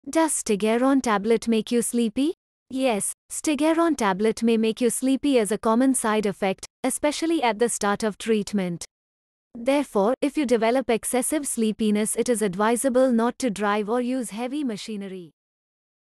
Q. Does Stugeron Tablet make you sleepy?
" Know more about Stugeron 25mg Tablet at /stugeron-25mg-tablet.
Yes, Stugeron Tablet may make you sleepy as a common side effect, especially at the start of treatment. Therefore, if you develop excessive sleepiness it is advisable not to drive or use heavy machinery."